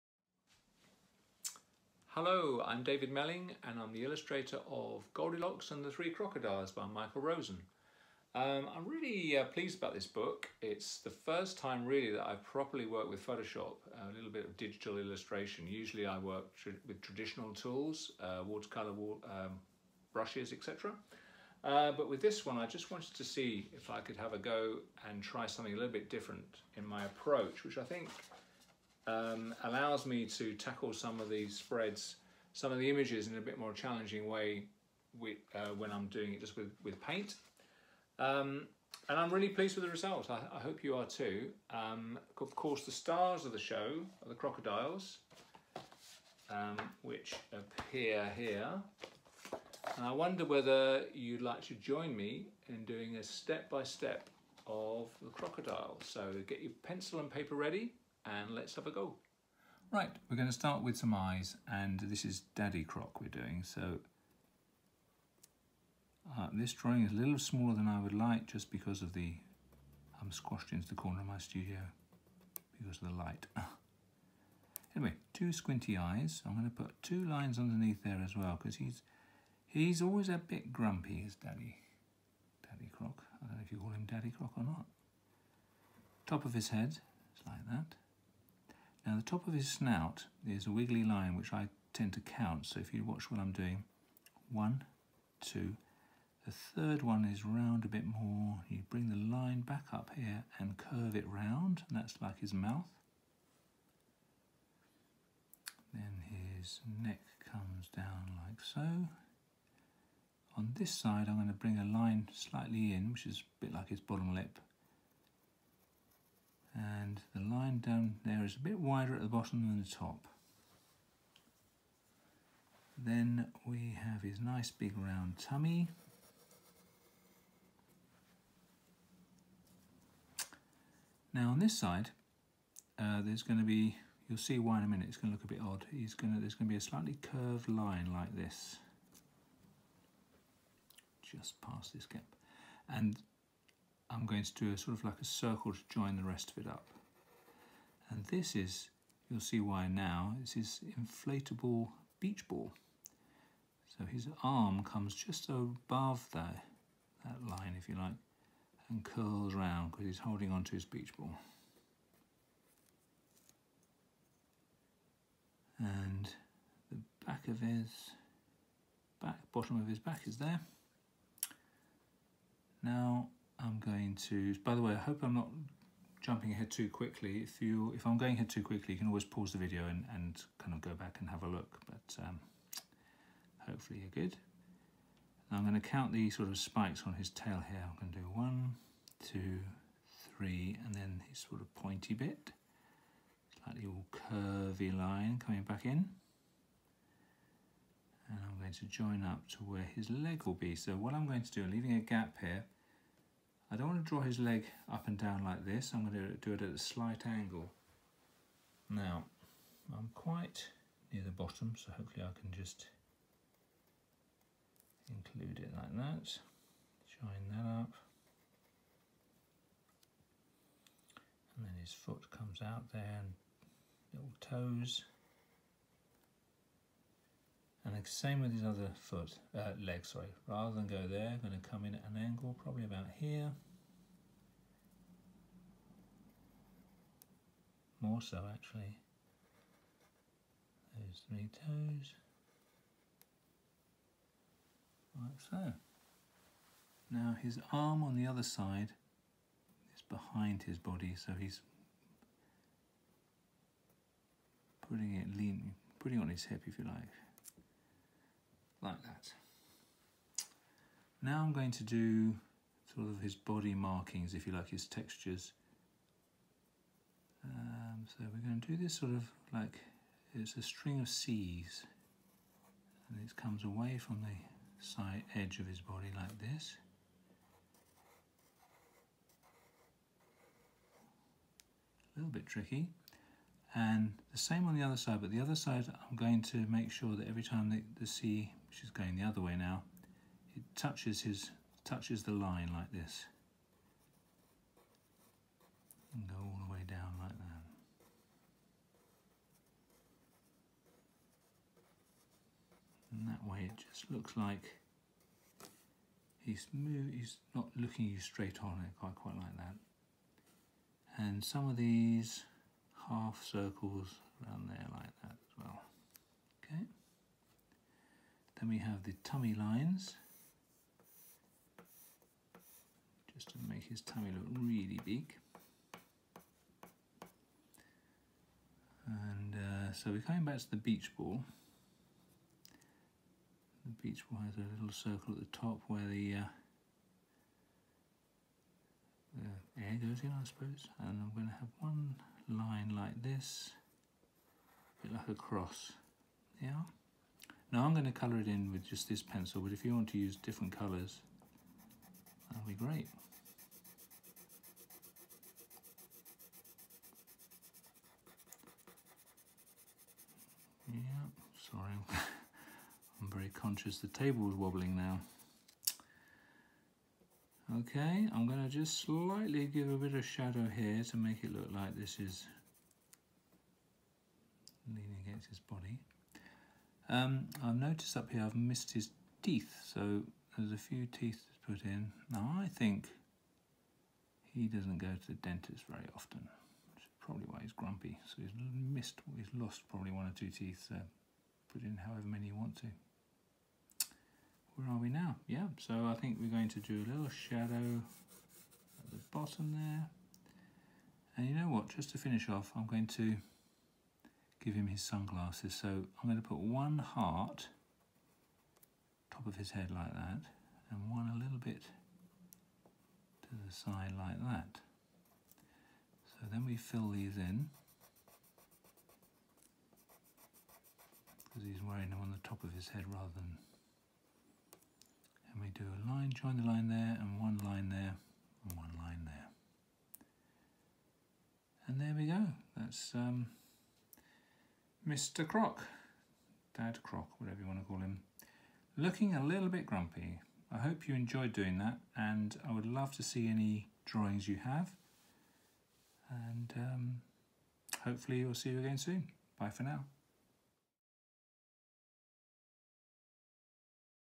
Goldilocks and the Three Crocodiles Draw-along with David Melling
Draw Daddy Croc along with David Melling, illustrator of the brand new picture book by Michael Rosen: Goldilocks and the Three Crocodiles – a fabulously funny take on a classic story from two giants of children’s books!